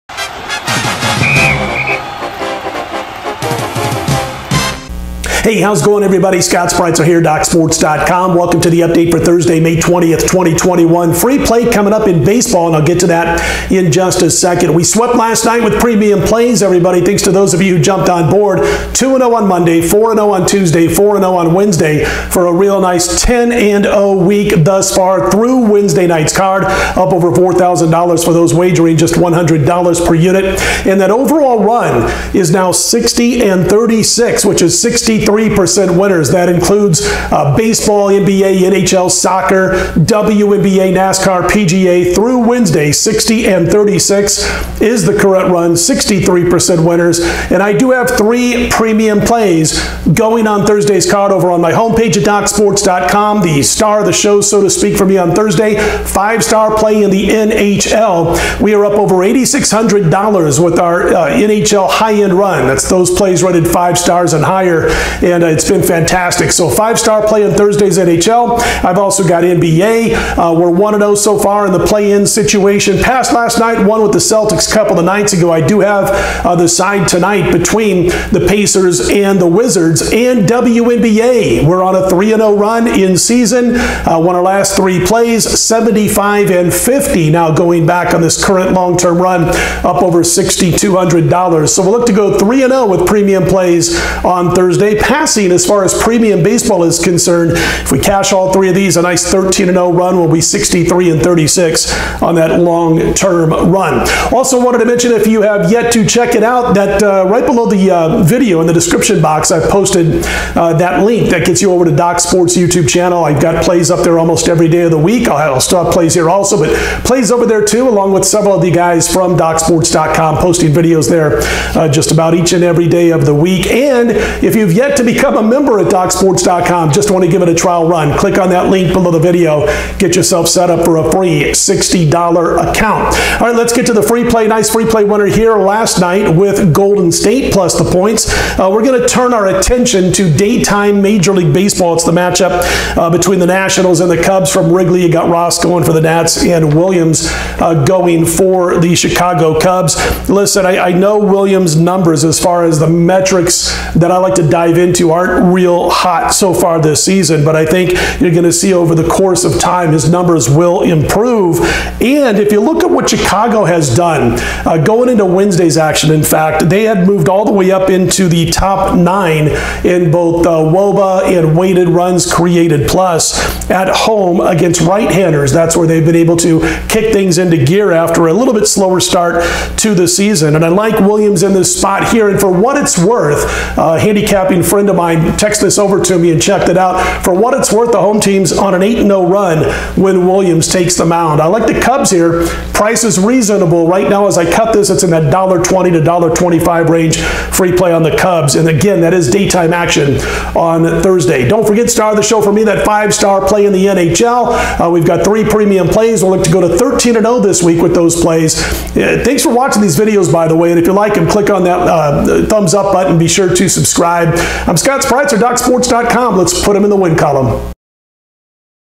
MLB Pick - Washington Nationals vs Chicago Cubs Prediction, 5/20/21, Free Betting Tips and Odds
Chicago Cubs vs Washington Nationals MLB picks and predictions 5/20/21. The Washington Nationals travel to Chicago, IL to face the Chicago Cubs at 2:15PM EST at Wrigley Field. Doc's Sports provides MLB picks, predictions, tips and MLB odds on this matchup of Washington Nationals vs Chicago Cubs